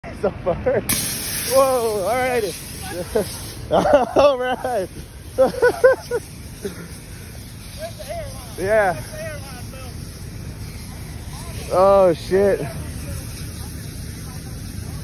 Trailer Fire! At Truck Stop
Guy connected jumper cables to his reefer and caught fire please be safe